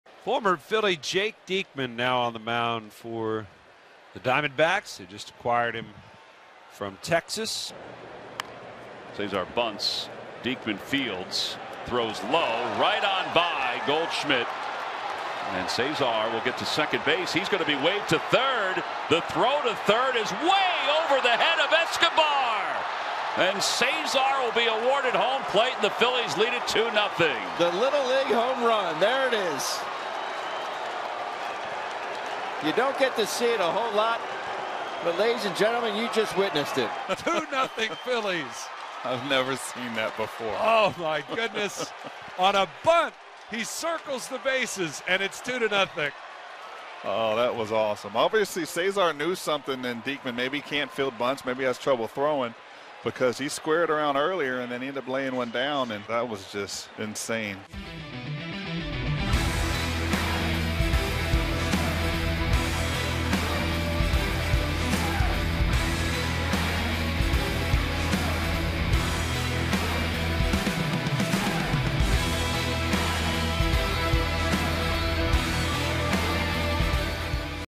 Cesar Hernandez bunts into a Little League HR
Phillies' Cesar Hernandez earns a Little League home run by dropping a bunt while running quickly out of the batter's box